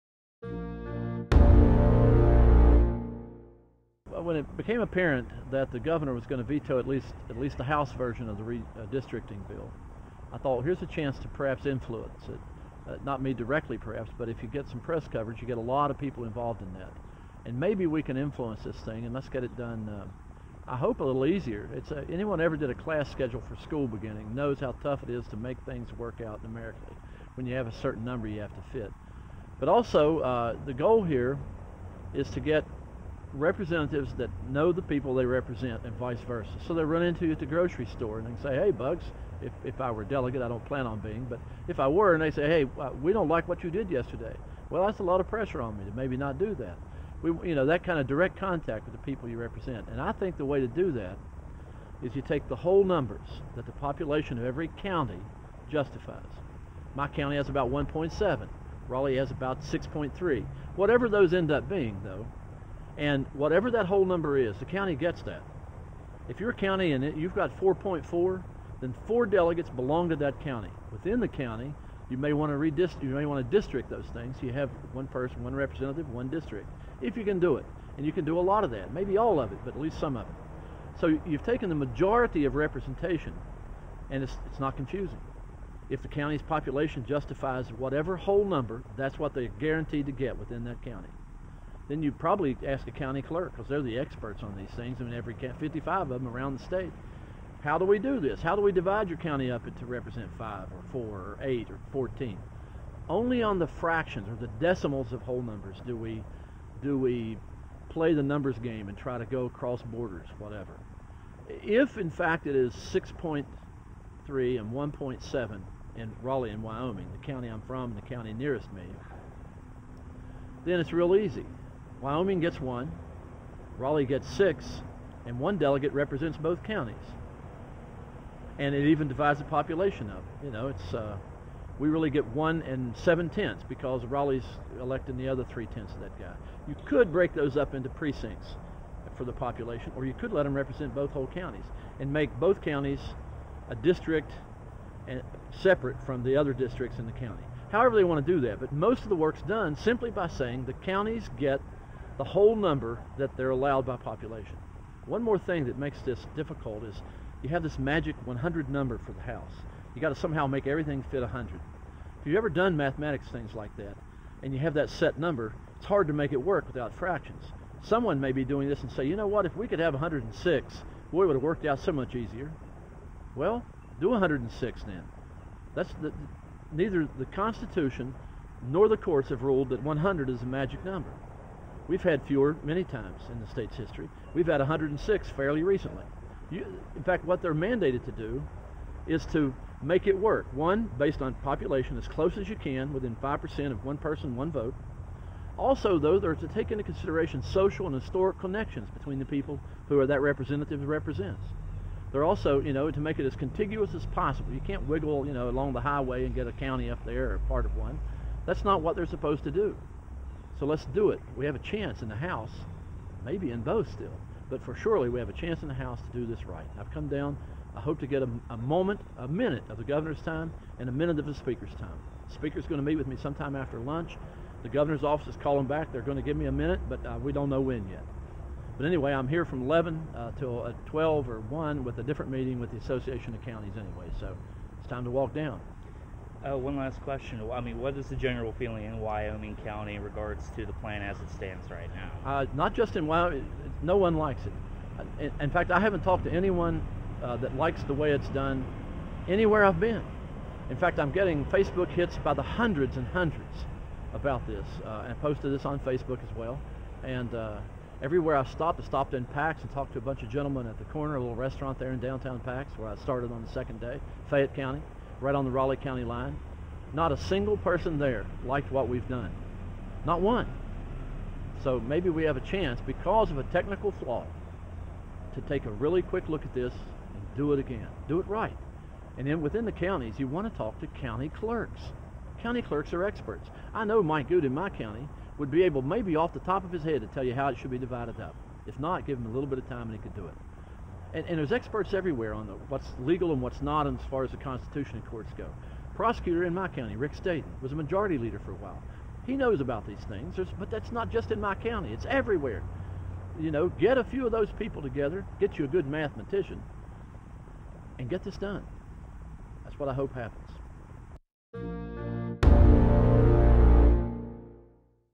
WVLEG: Wyoming County Circuit Clerk Brings Attention to Redistricting One Step at a Time
A Wyoming County official traveled to Charleston Tuesday morning to speak with officials in the West Virginia House of Delegates and the Acting Governor's office about the House redistricting plan. Not terribly unusual, except he walked 45 miles to do it.

David "Bugs" Stover, the Circuit Clerk of Wyoming County and president of the West Virginia Association of Circuit Clerks, walked to the State Capitol Complex from Beckley, W.Va.